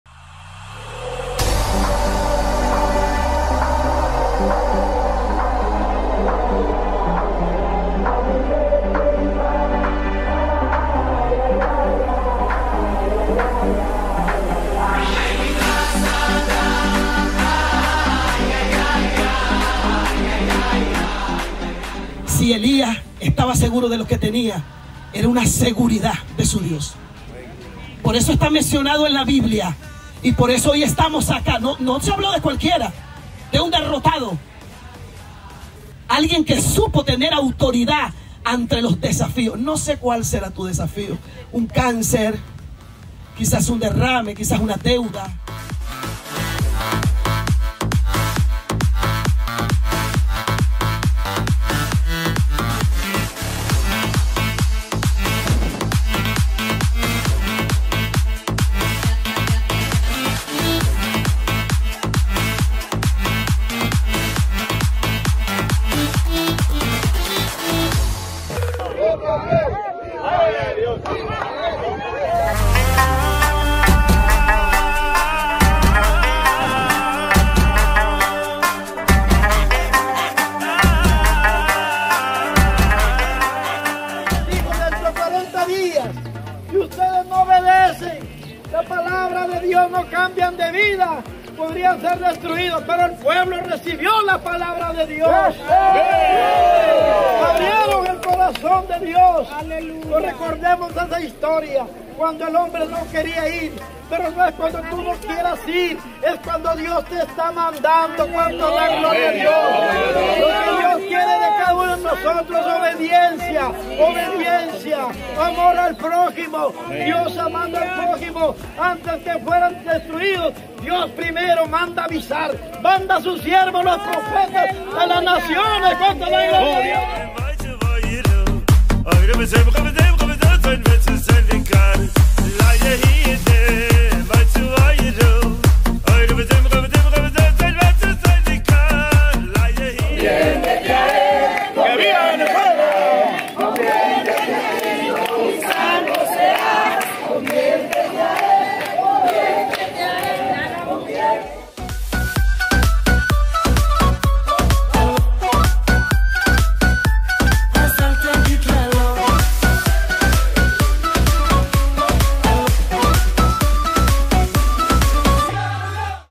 TRAS LAS HUELLAS DEL MESÍAS EN ISRAEL | DIA 3 | MAYO 2023
El tour "Tras las huellas del Mesías" con el Hno. Puertas. es un recorrido turístico por algunos lugares por donde caminó Jesús de Nazareth mientras estuvo en la tierra y algunos lugares emblemático que aparece en la biblia.

Asf fué el día 2 del Tour